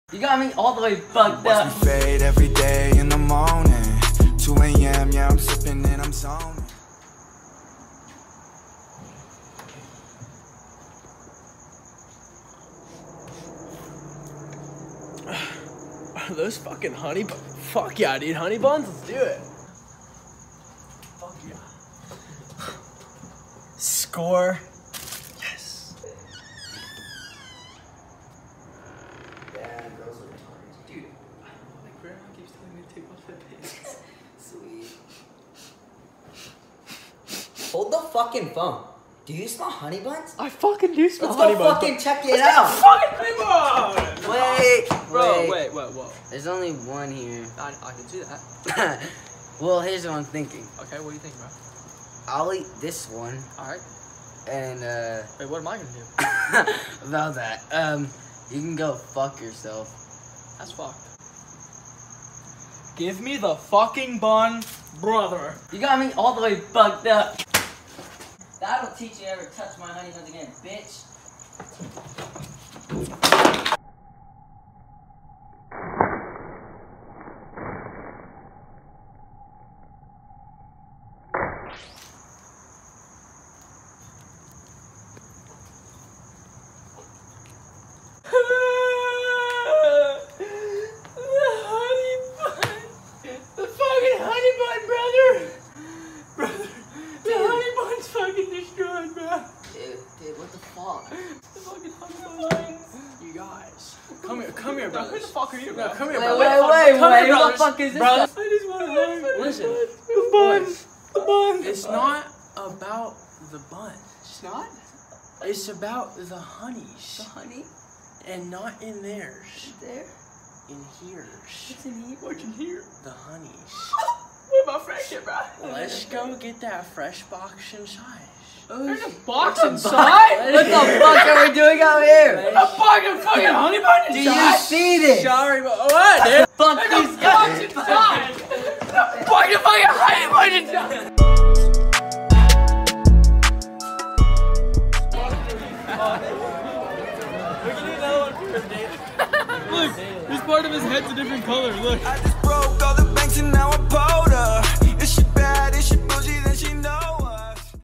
a very sad short film
This video is about honey buns being the last of their kind.